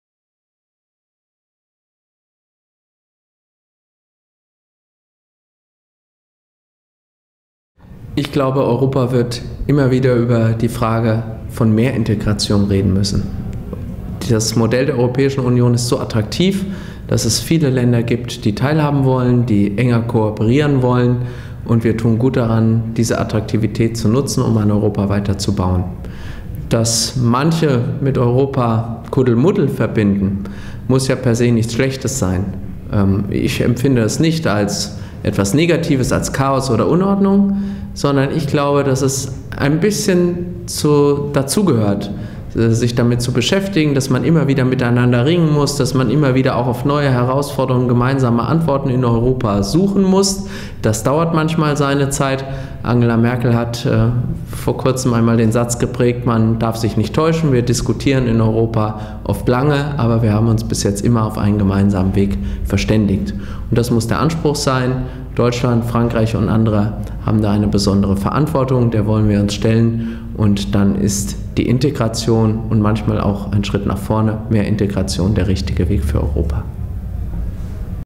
Peter Tauber
Video-Statement von Peter Tauber zum Thema Europas Kuddelmuddel  beim NZZ-Podium Berlin am 5. November 2015.